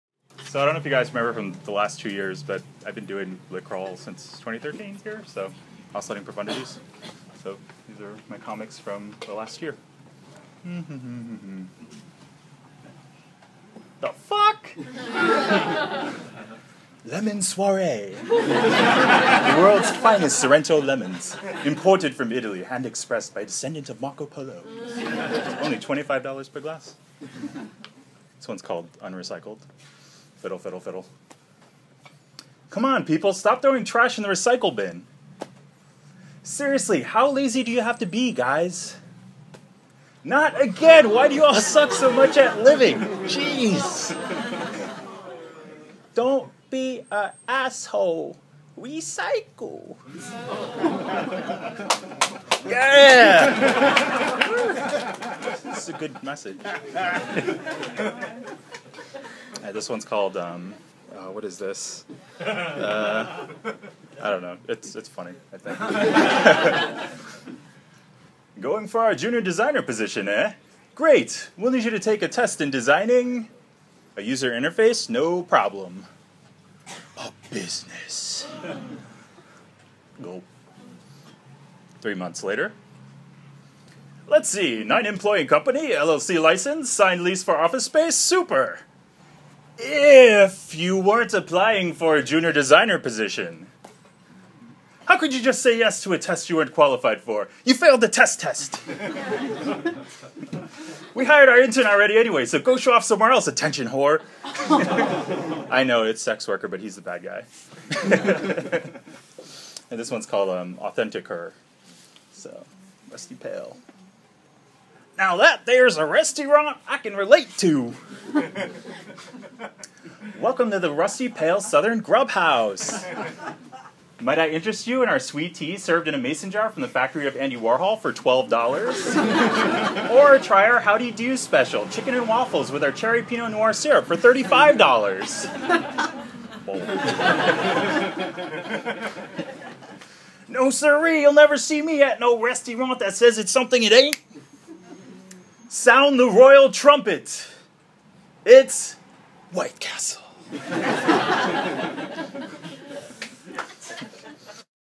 Oscillating Profundities - Live @Lit Crawl (part 1)
A few episodes of my webcomic, Oscillating Profundities read live for Lit Crawl 2015 @Mission Comics, San Francisco. Part 1 of 2.